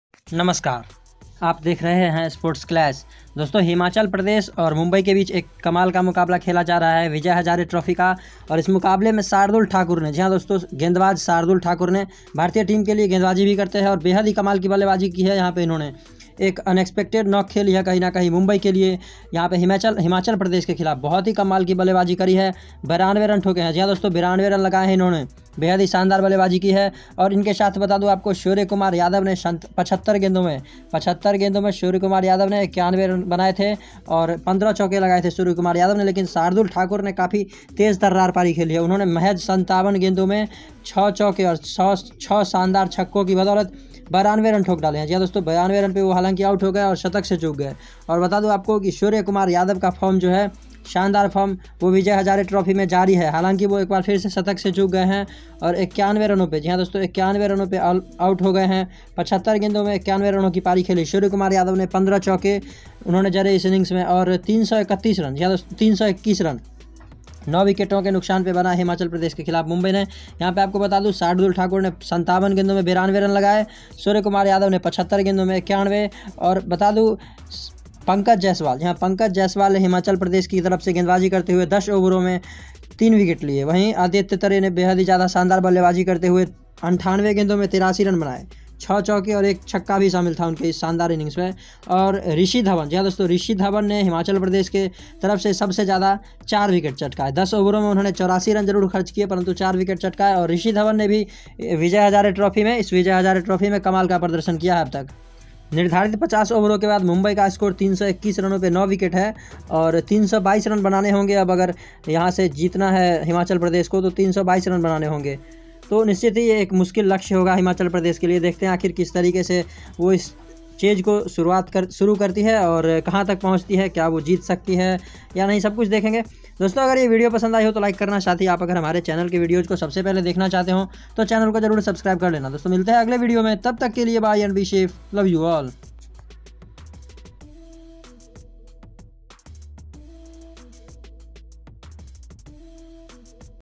Shardul Thakur 92 Vs Himachal Pradesh | Surya Kumar Yadav 91 Vs HIM | Vijay Hazare Trophy
surya kumar Yadav 91
shardul Thakur 92   vs Himachal Pradesh